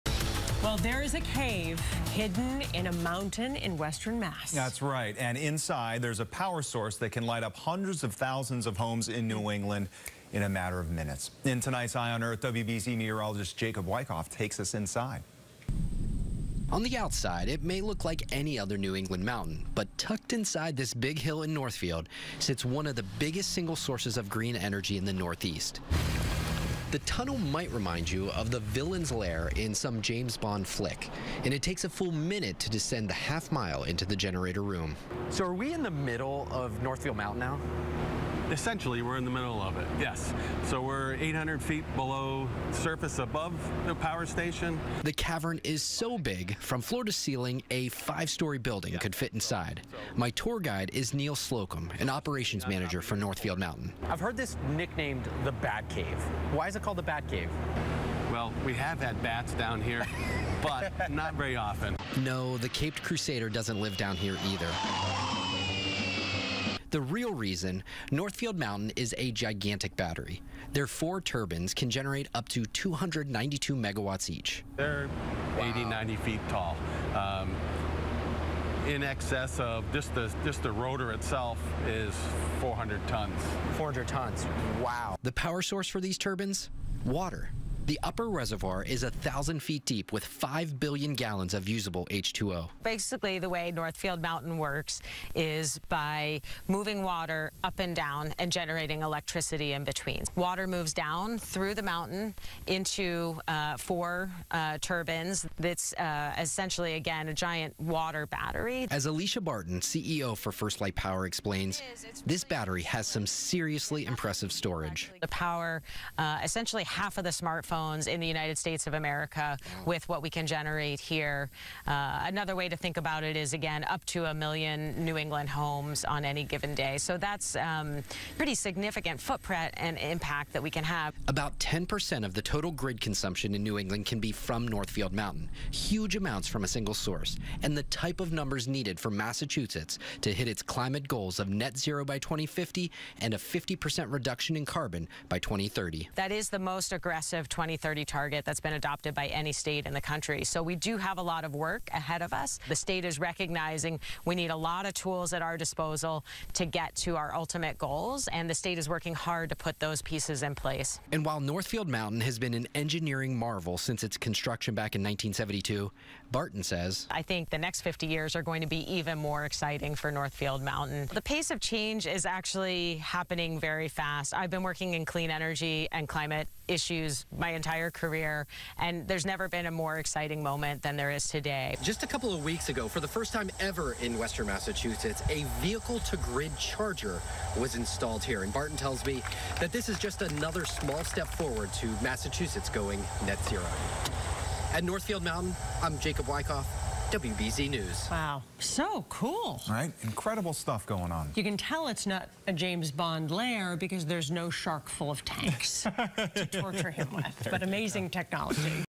Visiting New England's largest battery in the *MIDDLE* of Northfield Mountain
NORTHFIELD – There is a cave hidden in the hills of Northfield Mountain. Inside is a power source that can light up hundreds of thousands of homes in New England in a matter of minutes.

It may look like any normal lake, but this pond is so much more. At one time, the water at Northfield Mountain served as the world's largest battery.